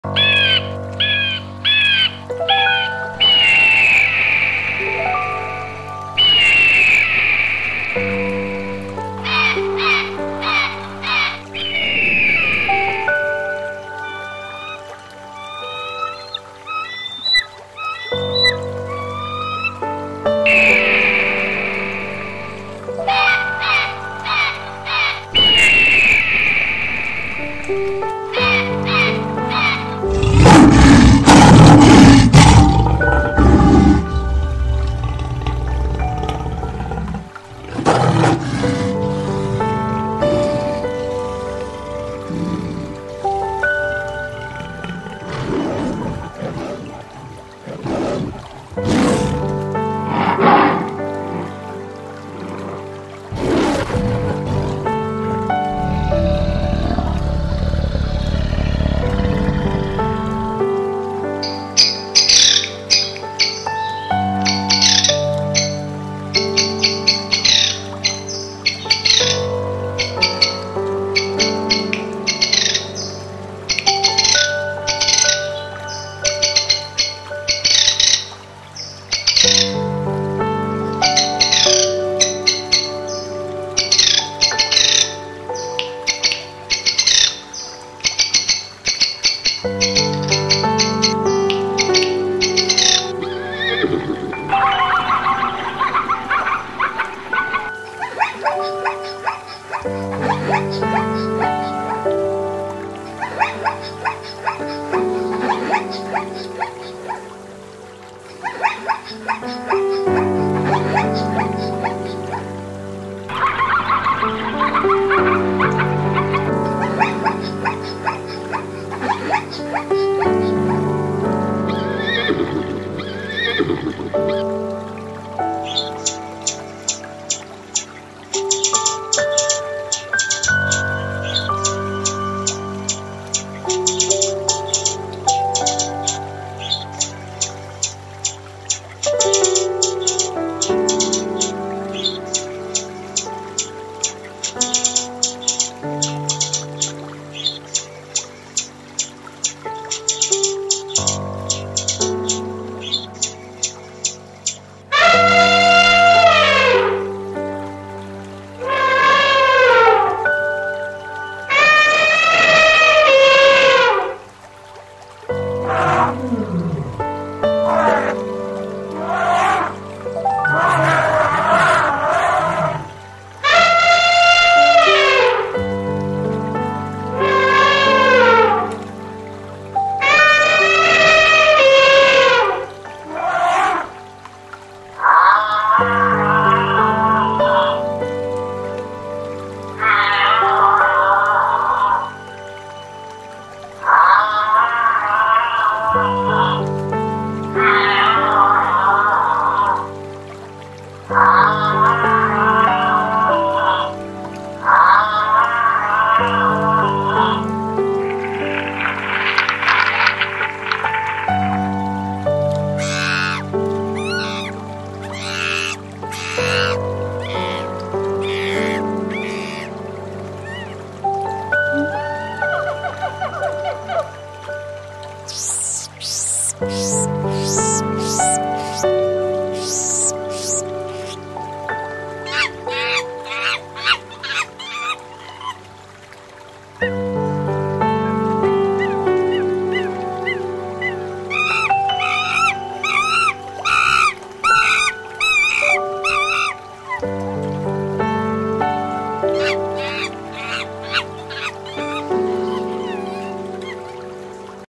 wild animal sounds: eagle, lion, zebra and others
In the wild there are many animal. There are those who can fly, predator, grass Eater, and omnivora. This video introduces some of these animal. Such as eagle, lion, woodpecker, zebra, hummingbird, elephant, giraffe and monkey